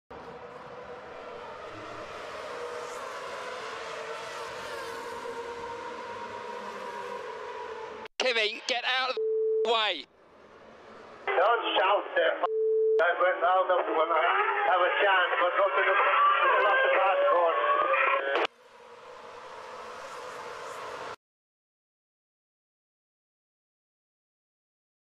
Kimi Räikkönen: Don't fucking shout... (Team radio) - Indian grand prix 2013 (10/27/2013)
Team radio:
Alan Permane: "Kimi, Get out of the fucking way!"
Kimi Räikkönen: ''Don't fucking shout..."